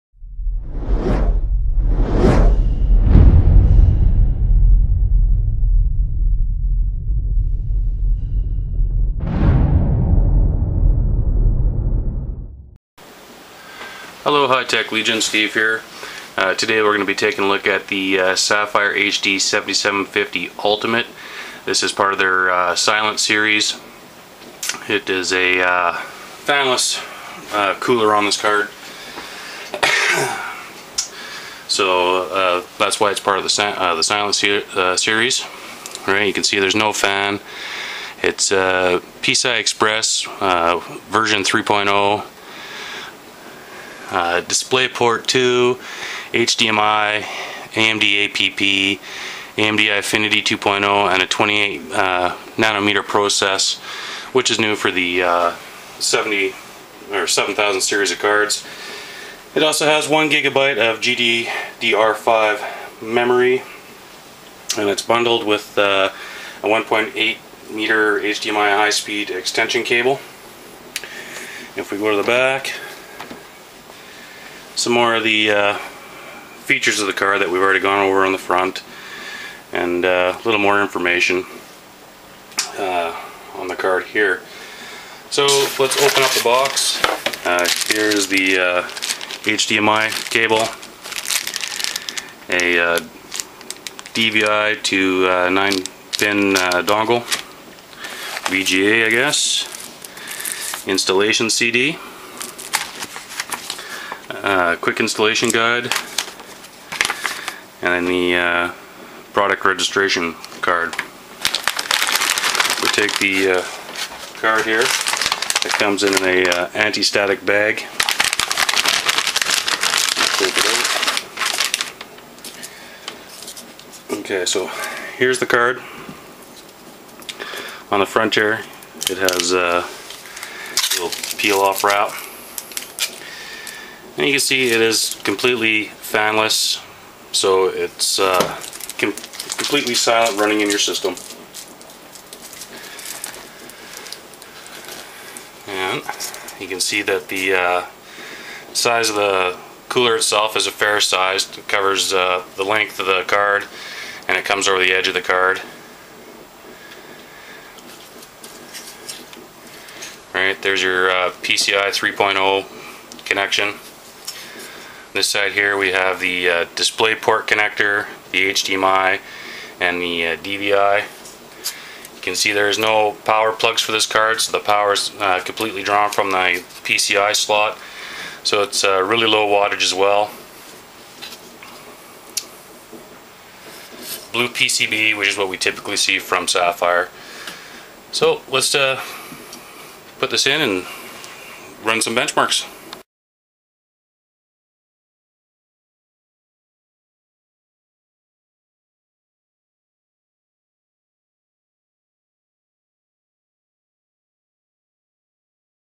Sapphire 7750 Ultimate Video Card Overview
The Sapphire HD 7750 Ultimate uses a dual-slot passive cooling system with zero moving parts. This makes the Sapphire HD 7750 Ultimate completely silent and perfect for a gaming HTPC or in a library/office setting. Underneath the hood of the Sapphire Radeon HD 7750 Ultimate is AMD's 3rd generation DirectX11 28nm GPU based on the Graphics Core Next Architecture, sporting an 800MHz core clock with 1GB of GDDR5 on a 128-bit memory bus. Like all AMD Radeon 7000 series cards, the Sapphire HD 7750 Ultimate comes with AMD HD3D support via HDMI 1.4a as well as AMD Eyefinity and next-generation 4K display support for the ultimate in gaming immersion.